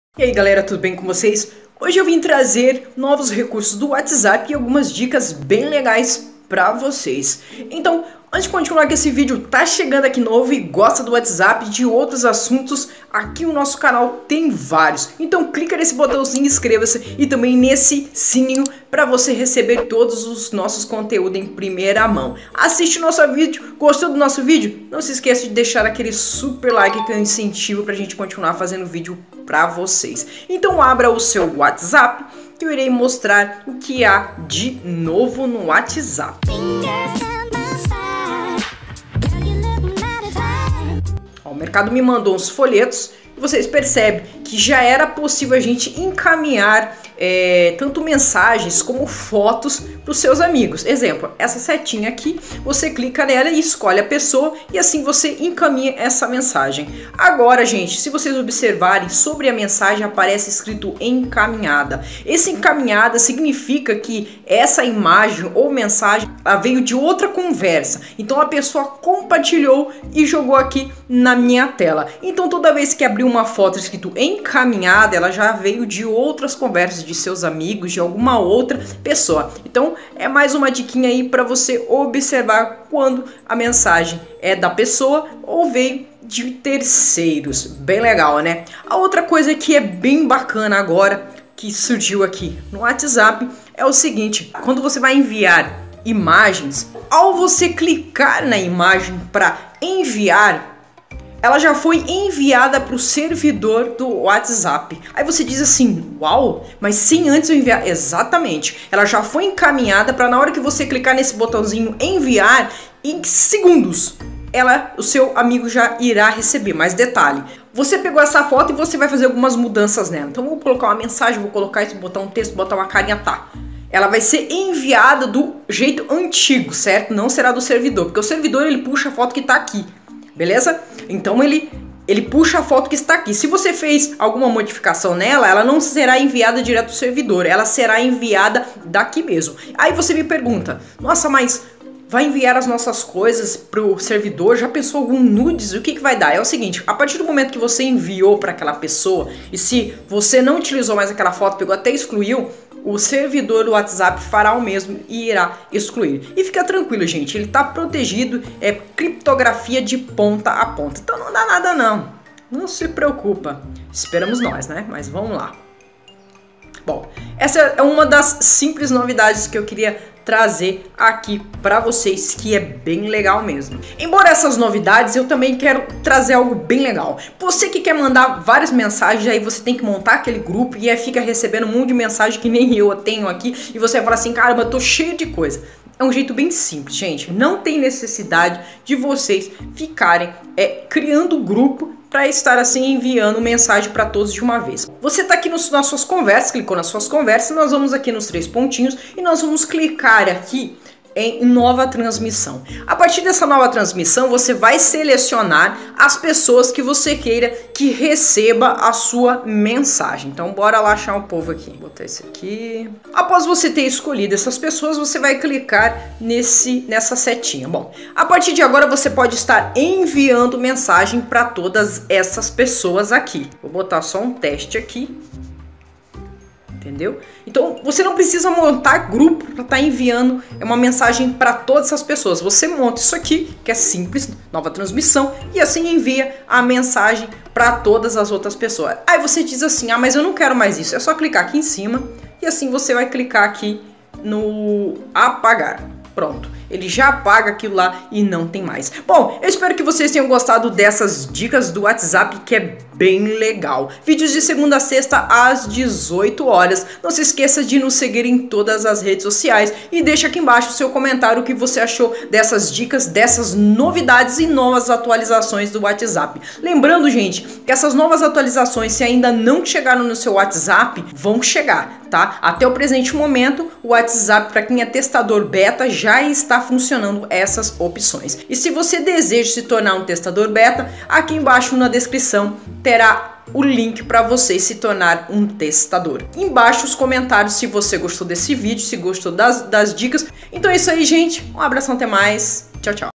Whatsapp Novidades e Dica
Whatsapp vem sempre atualizando e com isso novidades que vão deixar seu whatsapp melhor ainda.
A opcão encaminha mensagens,fotos ou vídeo já existe tem um tempo, mas agora é possível você saber se as fotos, mensagens ou vídeo foram encaminhado de outras pessoas.
Outra novidade que vai facilitar sua vida é o envio mais rápido de suas fotos, ao escolher uma foto, imediatamente é enviada para o servidor do whatsapp, sendo assim, ao você enviar em segundo a pessoa receberá. Não será mais demorado como antigamente.
E temos uma dica que vai ajudar ao seu dia a dia, ao enviar mensagem a várias pessoas do whatsapp é possível sem criar um grupo, enviar para todas de uma só vez.

E me acompanhe pra ficar por dentro de tudo.